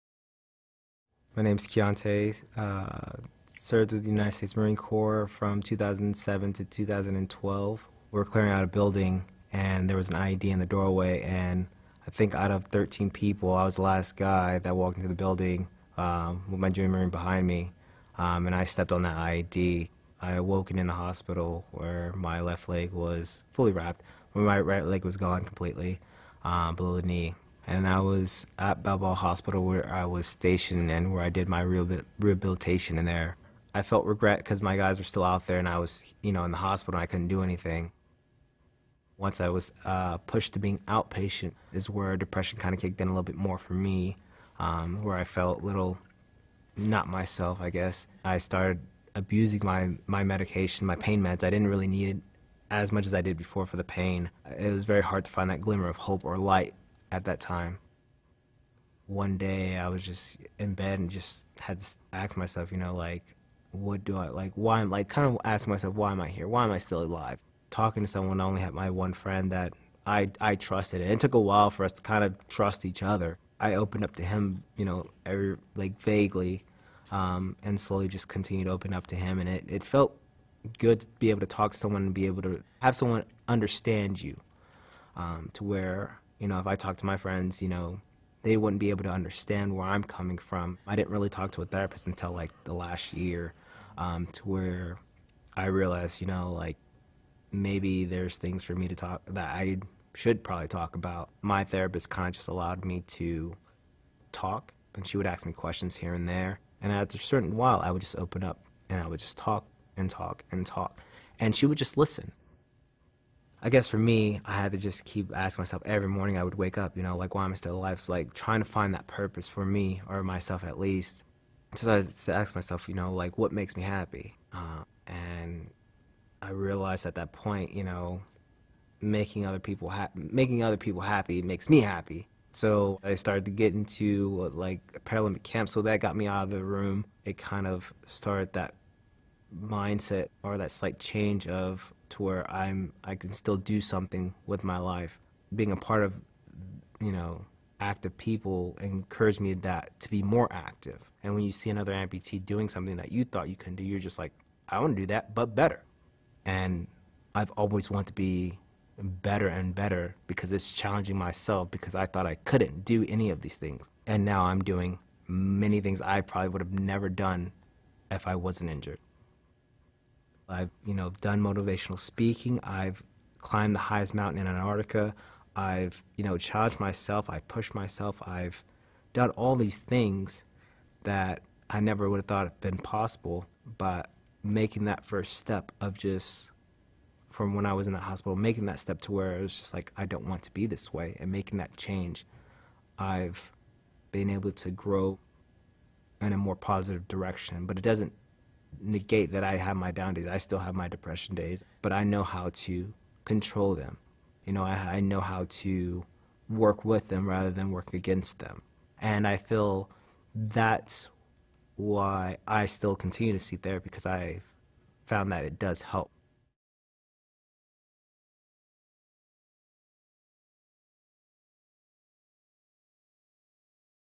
A Marine’s physical injury led to personal growth
Kionte, a Marine Veteran, faced his physical and mental health challenges head-on. Now, he doesn’t let his past hold him back.